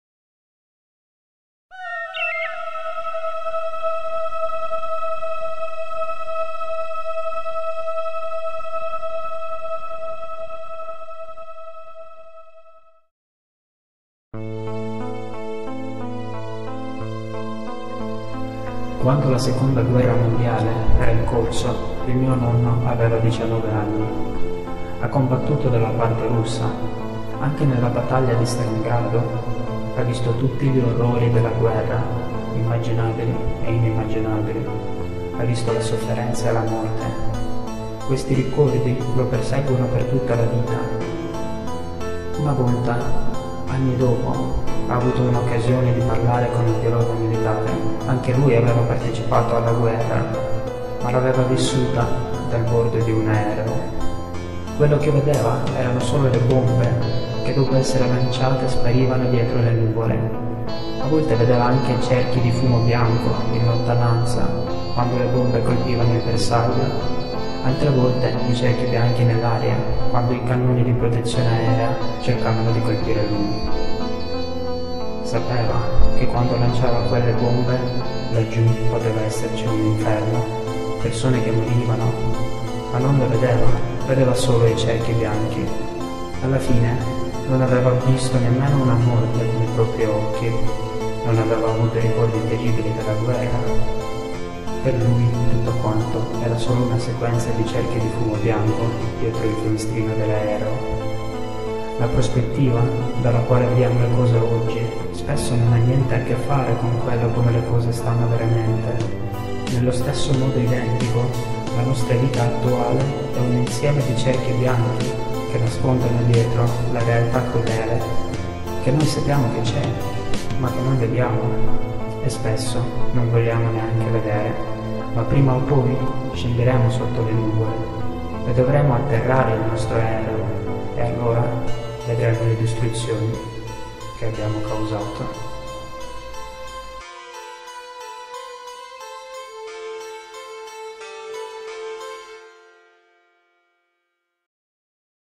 Sopra le nuvole
Quando la Seconda Guerra Mondiale era in corso il mio nonno aveva 19 anni, ha combattuto dalla parte russa anche nella battaglia di Stalingrado, ha visto tutti gli orrori della guerra immaginabili e inimmaginabili, ha visto la sofferenza e la morte. Questi ricordi lo perseguono per tutta la vita.
Una volta anni dopo ha avuto un'occasione di parlare con un pilota militare. Anche lui aveva partecipato alla guerra, ma l'aveva vissuta dal bordo di un aereo. Quello che vedeva erano solo le bombe che dopo essere lanciate sparivano dietro le nuvole, a volte vedeva anche cerchi di fumo bianco in lontananza quando le bombe colpivano il bersaglio, altre volte i cerchi bianchi nell'aria quando i cannoni di protezione aerea cercavano di colpire lui. Sapeva che quando lanciava quelle bombe, laggiù poteva esserci un inferno, persone che morivano, ma non le vedeva... vedeva solo i cerchi bianchi. Alla fine non aveva visto nemmeno una morte con i propri occhi, non aveva avuto ricordi terribili della guerra, per lui tutto quanto era solo una sequenza di cerchi di fumo bianco dietro il finestrino dell'aereo.
La prospettiva dalla quale vediamo le cose oggi spesso non ha niente a che fare con quello come le cose sono veramente. Nello stesso modo identico la nostra vita attuale è un insieme di cerchi bianchi che nascondono dietro la realtà crudele che noi sappiamo che c'è ma che non vediamo e spesso non vogliamo neanche vedere. Ma prima o poi scenderemo sotto le nuvole e dovremo atterrare il nostro aereo e allora vedremo le distruzioni che abbiamo causato.

Sergey Kutsenko